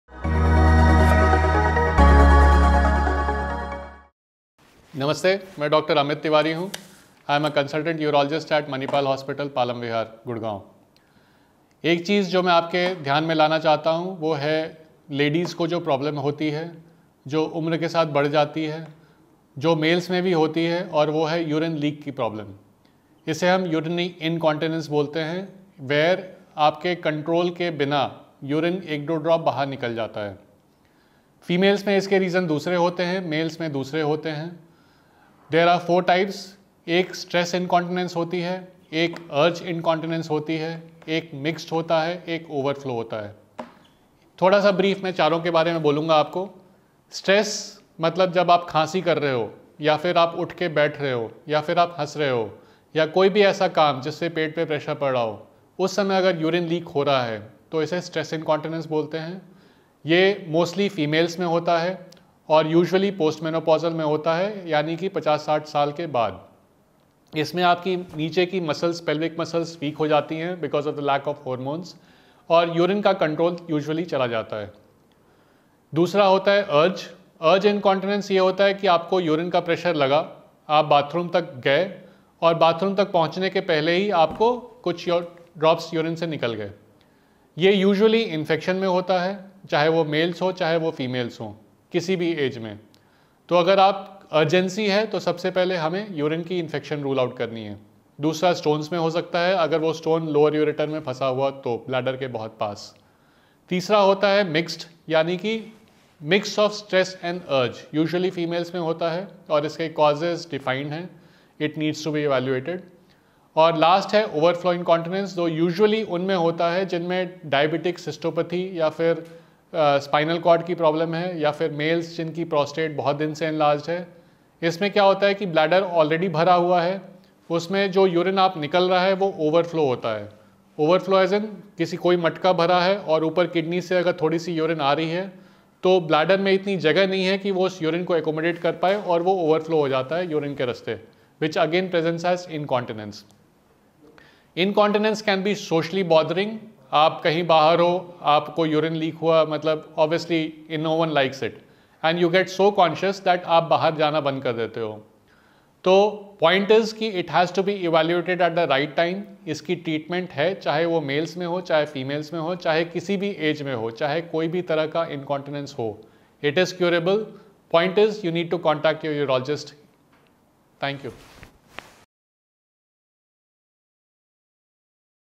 Urinary Incontinence | Dr. Amit Tiwari | Manipal Hospital Gurugram
Urinary incontinence, the loss of bladder control, is a common and often embarrassing problem. The severity ranges from occasionally leaking urine when you cough or sneeze to having an urge to urinate that's so sudden and strong you don't get to a toilet in time. Watch this video by Dr. Amit Tiwari, Consultant Urologist, Manipal Hospital, Gurugram, to know about the factors that cause this condition.